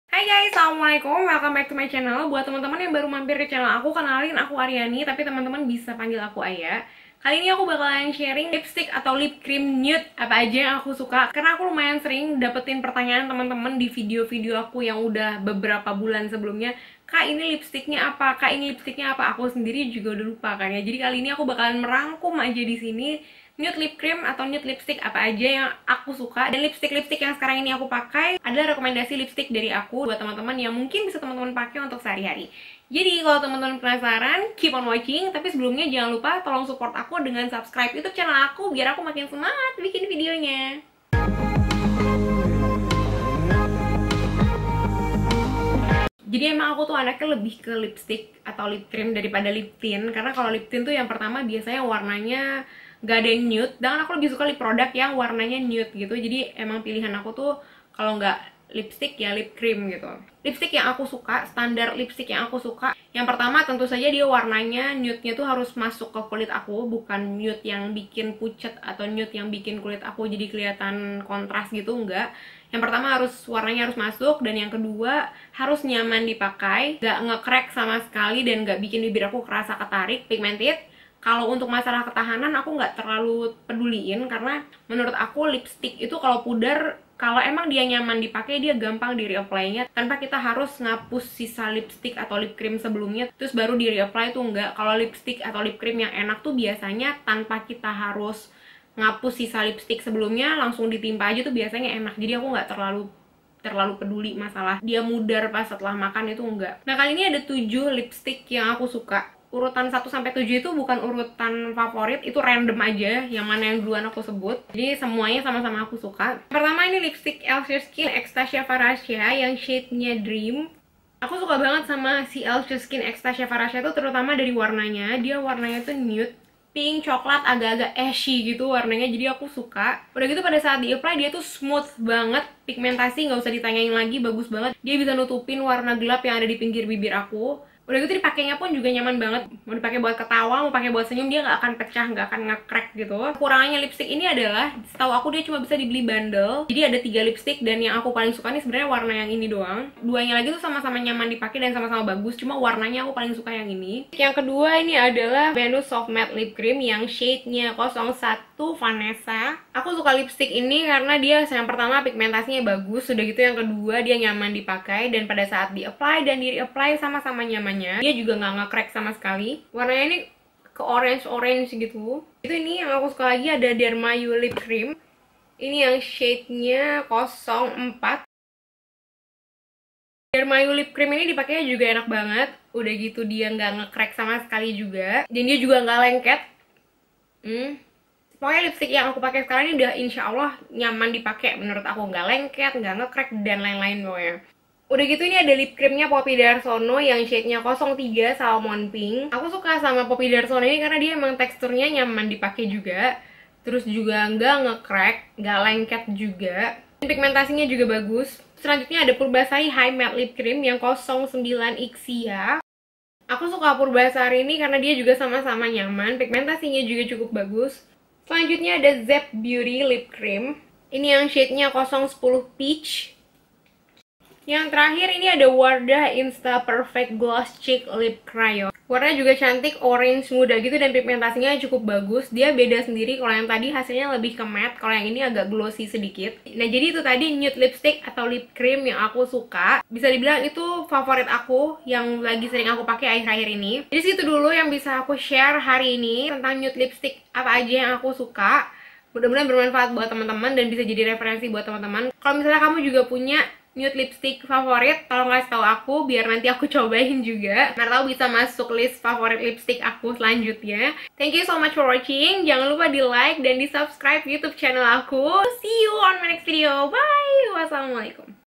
NUDE LIPSTICK LOKAL FAVORITE
Nude lipstick lokal favorite yang aku mention:

1. Elshe Skin x Tasya Farasya
Dream
beli di : official store Tokopedia ElsheSkin
Harga : Rp 199.000,- (isi 3 lipstik)

2. Venus Soft Matte Lip Cream
Shade : 01 Vanesa
Beli di JD.ID

3. Dermayu Lip Cream
Shade: 04 Prety Woman
Harga : Rp 75.000,- (sekarang kayaknya lagi diskon jadi 35rb, ga tau sampai kapan)

4.Poppy Dharsono Liquefied Matte Lip Color
Shade : Salmon Pink
beli di: @poppydharsonocosmetics

5. Purbasari Hi Matte Lip Cream
Shade : Ixia
Beli di : Shopee
Harga : sekitar Rp35.000,-

6. Zap Beauty Lip Cream
Shade : 010 Peach
Beli di : ZAP Clinic
Harga : 150.000,-

7. Wardah Instaperfect Gloss Lip Crayon